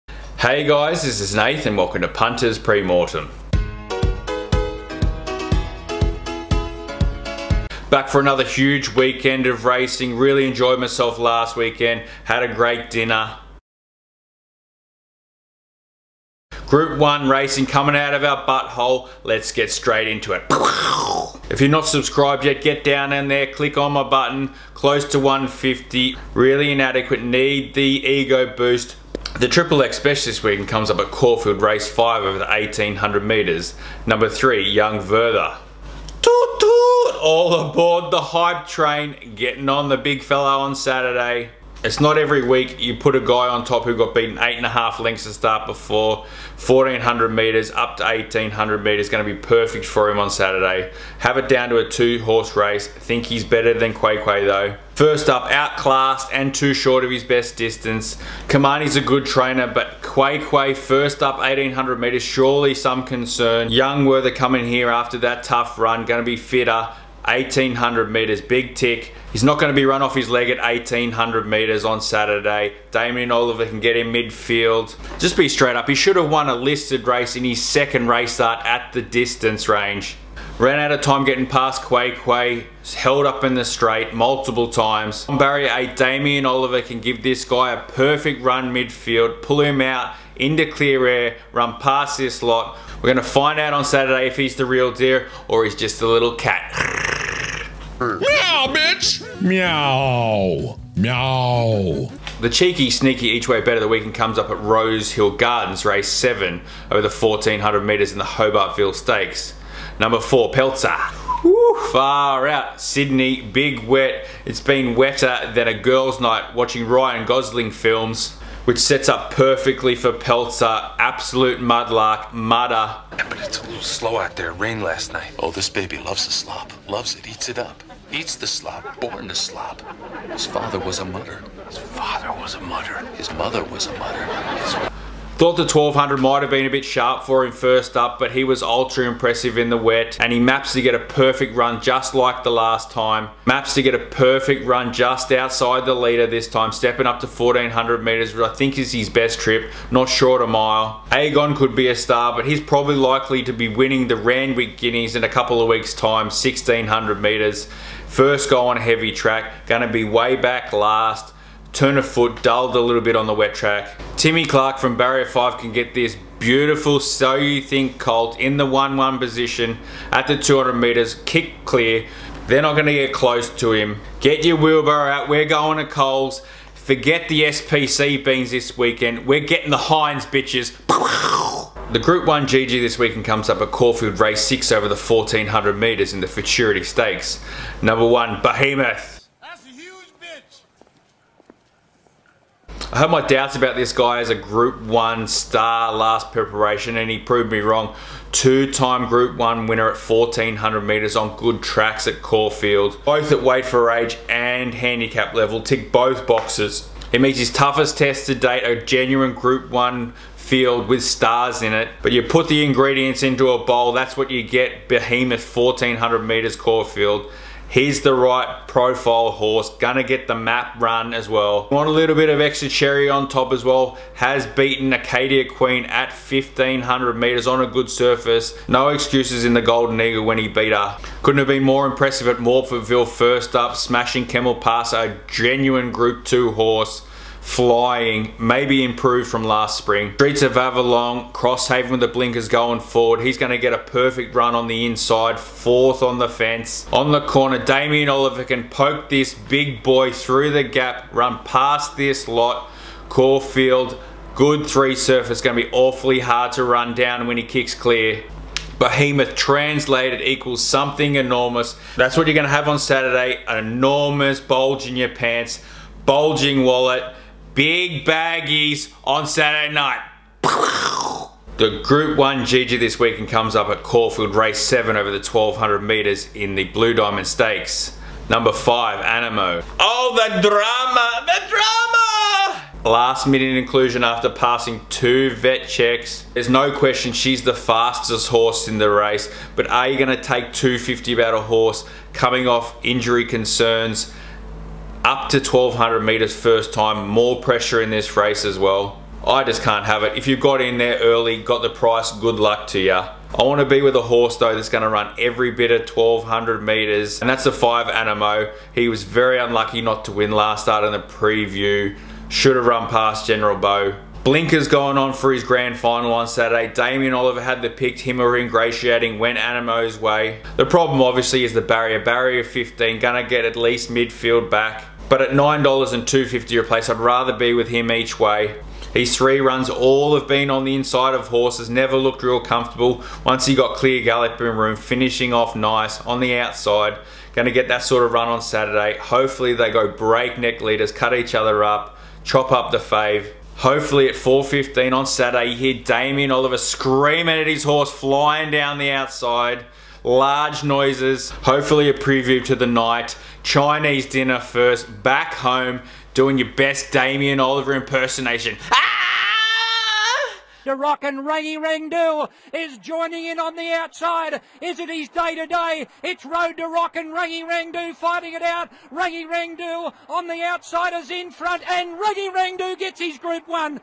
Punters Pre Mortem (PPM) #152 Blue Diamond Stakes Day February 20th 2021 Caulfield
XXXSPEC : Young Werther ($3.50) Caulfield Race 5 #3
Sneaky Eachy : Peltzer ($4) Rosehill Race 7 #4
G1GG: Behemoth ($3.50) Caulfield Race 6 #1
G1GG: Anamoe ($9) Caulfield Race 7 #5

XXXSPEC : Kolding ($5) Randwick Race 7 #1 SOFT 5 or BETTER ONLY
Sneaky Eachy : Roheryn ($7) Randwick Race 6 #4 MEOWWW
G1GG: Bivoauc ($2.80) Flemington Race 8 #2 CAT

Big love,
Nath.